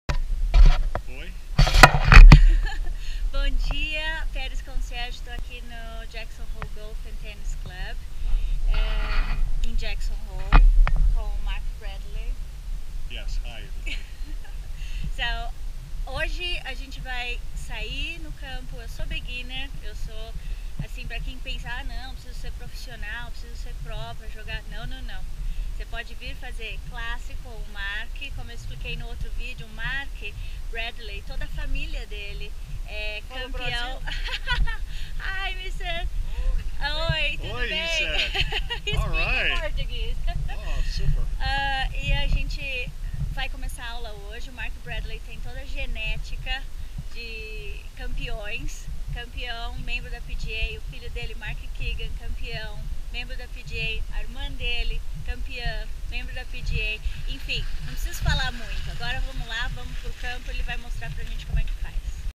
Dia de Golf com Mark Bradley, campeao e membro da PGA
Hoje a Patys Concierge iniciou suas aulas de golf com um dos mais renomados campeos e membro da PGA, Mark Bradley, pai do Keegan Bradley, que foi campeao da PGA Tour em 2011 e esta no ranking dos melhores jogadores de golf da PGA e do mundo